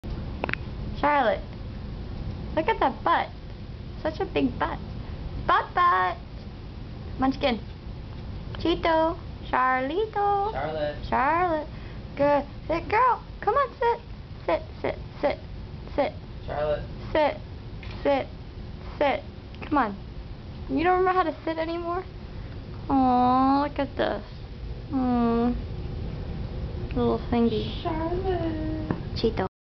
Visiting Charlotte
visiting the furry monster at the animal hospital. She's a trooper i tell you